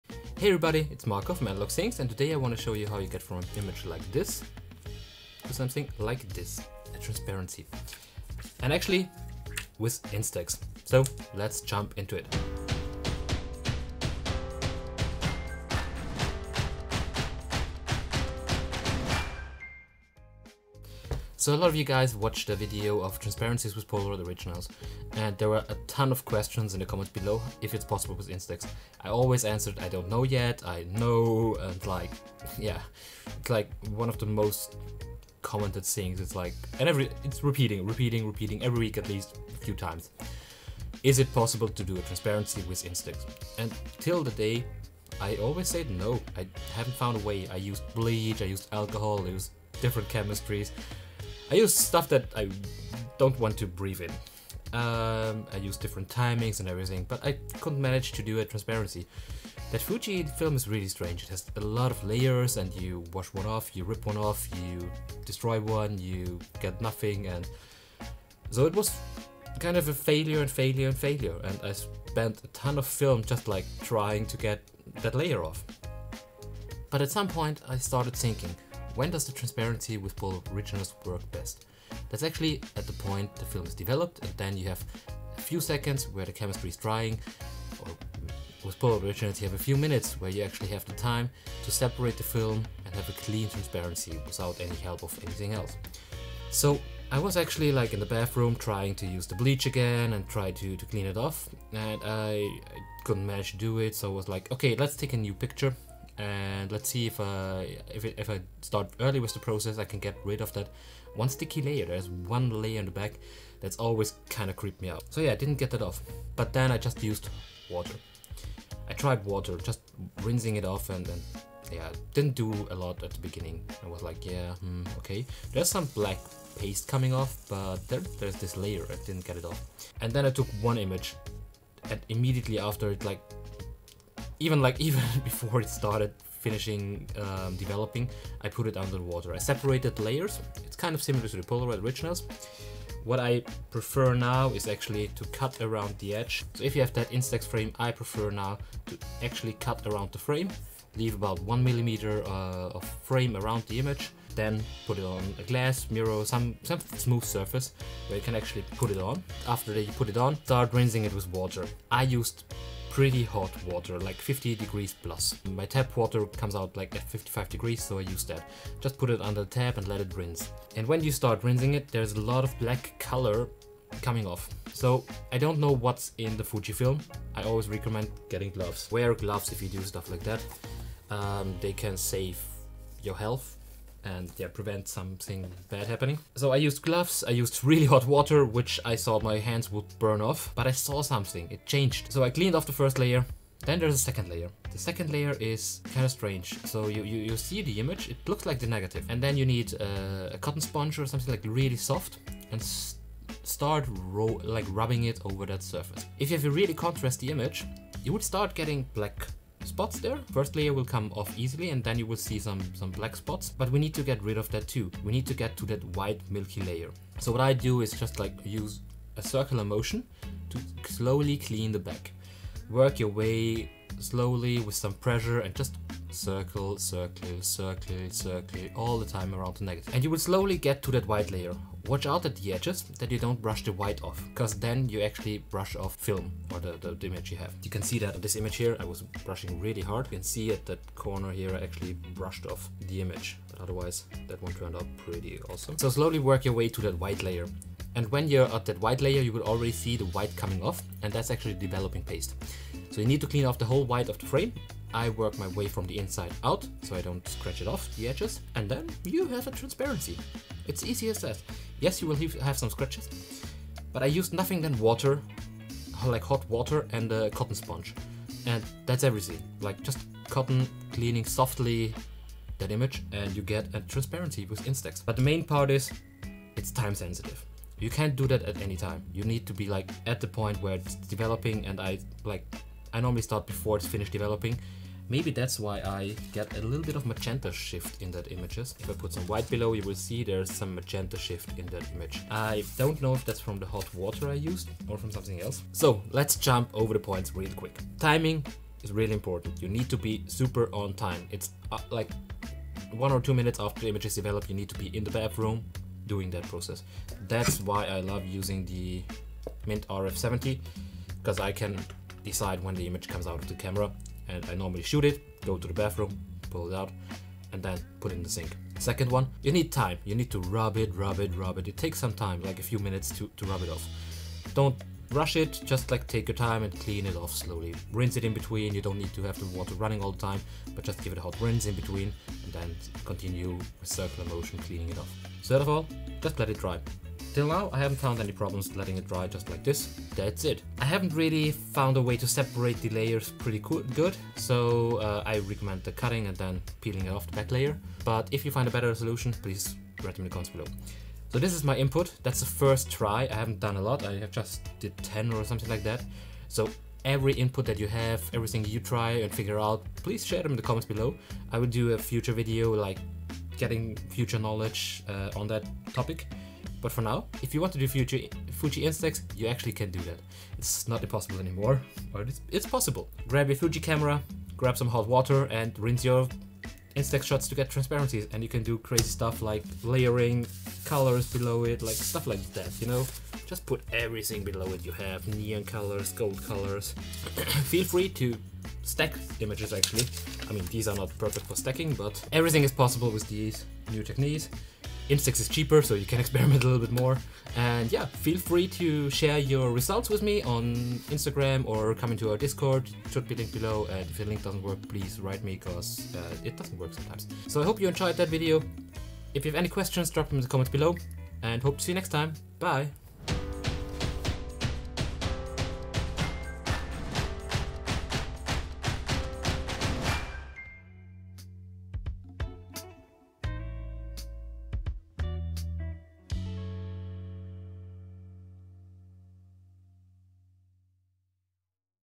FUJI INSTAX TRANSPARENCY, be creative with INSTAX
Up to this day i always thought that a transparency with Fujifilm Instax was not possible, but i proofed myself to be wrong. Maybe i was overthinking the whole process but it's a simple solution that everyone can do at home.

Follow the rules and behave.